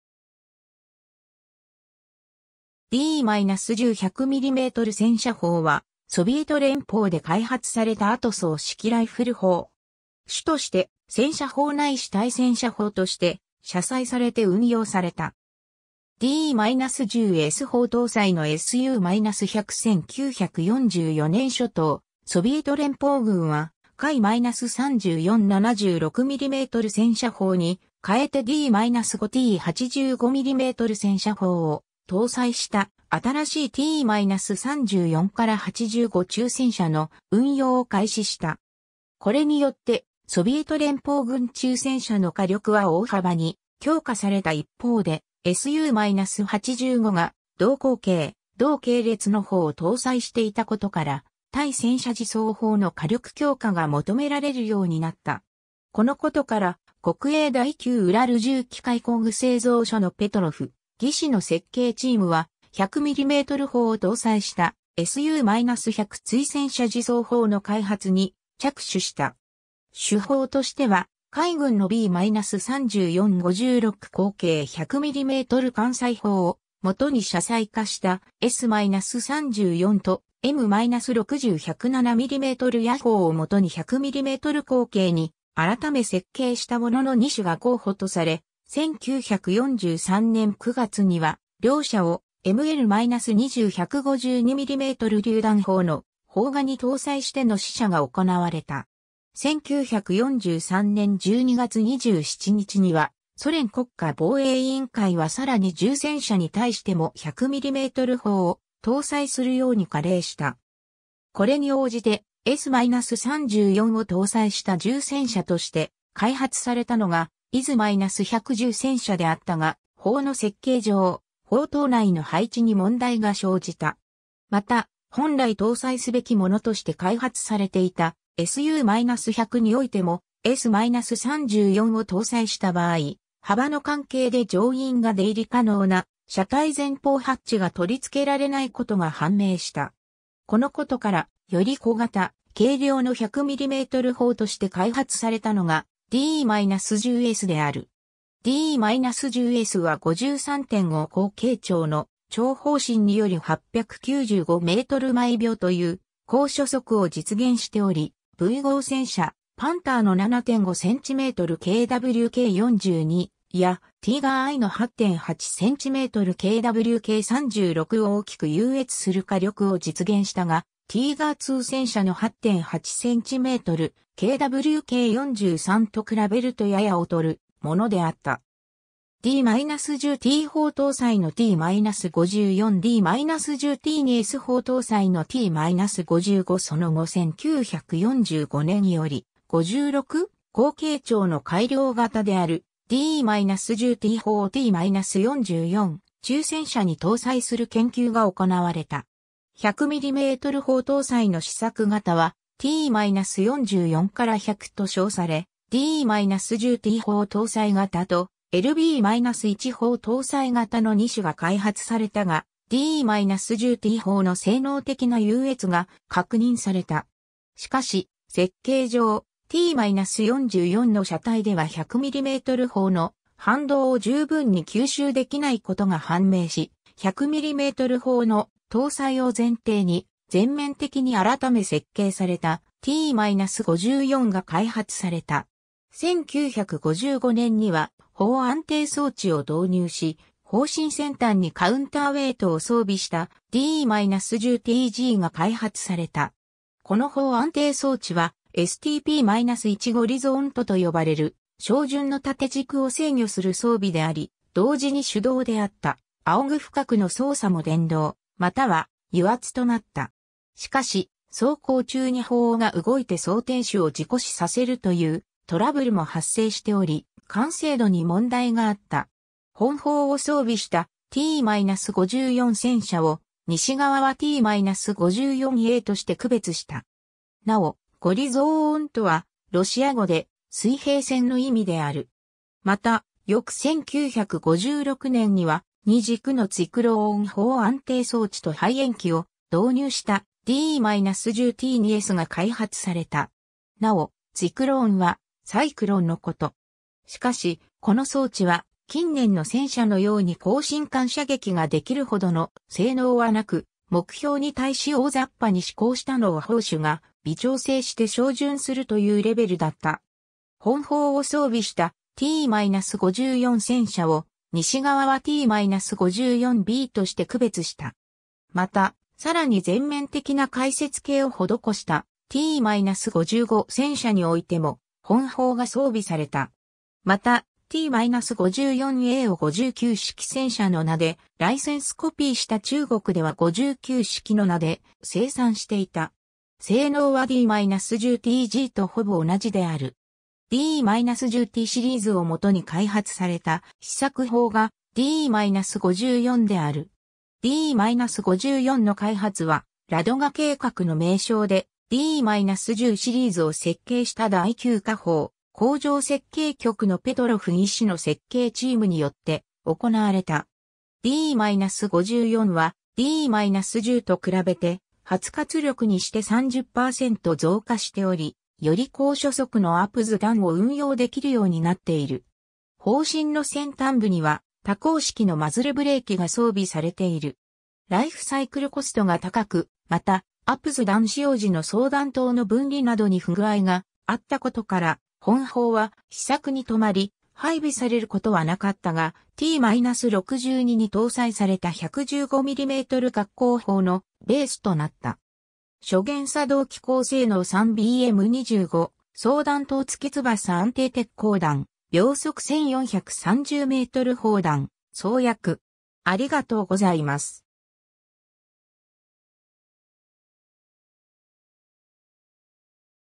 D-10 100mm戦車砲
D-10 100mm戦車砲は、ソビエト連邦で開発された後装式ライフル砲。
主として戦車砲ないし対戦車砲として、車載されて運用された。
 D-10S砲搭載のSU-100 1944年初頭、ソビエト連邦軍は、F-34 76mm戦車砲に換えてD-5T 85mm戦車砲を搭載した、新しいT-34-85中戦車の運用を開始した。
これによってソビエト連邦軍中戦車の火力は大幅に強化された一方で、SU-85が同口径・同系列の砲を搭載していたことから、対戦車自走砲の火力強化が求められるようになった。
 このことから、国営第9ウラル重機械工具製造所（OKB No.9 UZTM）のペトロフ技師の設計チームは、100mm砲を搭載したSU-100対戦車自走砲の開発に着手した。
 主砲としては、海軍のB-34 56口径100mm艦載砲を元に車載化したS-34と、M-60 107mm野砲を元に100mm口径に改設計したものの2種が候補とされ、1943年9月には、両者をML-20 152mm榴弾砲の砲架に搭載しての試射が行われた。
1943年12月27日には、ソ連国家防衛委員会（GKO）はさらに重戦車に対しても100mm砲を搭載するように下令した。
これに応じて、S-34を搭載した重戦車として開発されたのがIS-100重戦車であったが、砲の設計上、砲塔内の配置に問題が生じた。
また、本来搭載すべきものとして開発されていたSU-100においても、S-34を搭載した場合、幅の関係で乗員が出入り可能な車体前方ハッチが取り付けられないことが判明した。
 このことから、より小型・軽量の100mm砲として開発されたのがD-10Sである。
D-10Sは53.5口径長の長砲身により895m/秒という高初速を実現しており、V号戦車パンターの7.5 cm KwK 42やティーガーIの8.8 cm KwK 36を大きく優越する火力を実現したが、ティーガーII戦車の8.8 cm KwK 43と比べるとやや劣るものであった。
 D-10T砲搭載のT-54 D-10T2S砲搭載のT-55 その後1945年より、56口径長の改良型であるD-10T砲をT-44中戦車に搭載する研究が行われた。
100mm砲搭載の試作型はT-44-100と称され、D-10T砲搭載型とLB-1砲搭載型の2種が開発されたが、D-10T砲の性能的な優越が確認された。
 しかし、設計上、T-44の車体では100mm砲の反動を十分に吸収できないことが判明し、100mm砲の搭載を前提に全面的に改設計されたT-54が開発された。
 1955年には、砲安定装置を導入し、砲身先端にカウンターウェイトを装備したD-10TGが開発された。
この砲安定装置はSTP-1 ゴリゾーントと呼ばれる照準の縦軸を制御する装備であり、同時に手動であった仰俯角の操作も電動または油圧となった。
しかし、走行中に砲尾が動いて装填手を事故死させるというトラブルも発生しており、完成度に問題があった。
本砲を装備したT-54戦車を、西側はT-54Aとして区別した。
 なお、「ゴリゾーント」（горизонт ガリゾーント）は、ロシア語で「水平線」の意味である。
 また、翌1956年には、2軸のツィクローン砲安定装置と排煙器を導入したD-10T2Sが開発された。
なお、「ツィクローン」（циклон ツィクローン）は「サイクロン」のこと。
しかし、この装置は、近年の戦車のように行進間射撃ができる程の性能はなく、目標に対し大雑把に指向したのを砲手が微調整して照準するというレベルだった。
 本砲を装備したT-54戦車を、西側はT-54Bとして区別した。
また、さらに全面的な改設計を施したT-55戦車においても、本砲が装備された。
 また、T-54Aを59式戦車の名でライセンスコピーした中国では59式の名で生産していた。
性能はD-10TGとほぼ同じである。
 D-10Tシリーズをもとに開発された試作砲がD-54である。
D-54の開発は、ラドガ計画の名称で、D-10シリーズを設計した第9火砲工場設計局（旧：国営第9ウラル重機械工具製造所）のペトロフ技師の設計チームによって行われた。
 D-54は、D-10と比べて初活力にして30%増加しており、より高初速のAPDS弾を運用できるようになっている。
砲身の先端部には多孔式のマズルブレーキが装備されている。
ライフサイクルコストが高く、また、APDS弾使用時の装弾筒の分離などに不具合があったことから、...